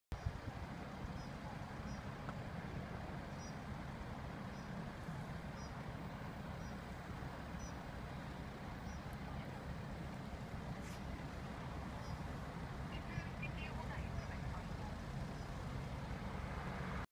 Collision between car and cycle on North Sea Lane, Cleethorpes.
Police at the scene of a collision betweenacar and a cycle, on Northe Sea Lane, Cleethorpes. Road closures are in place.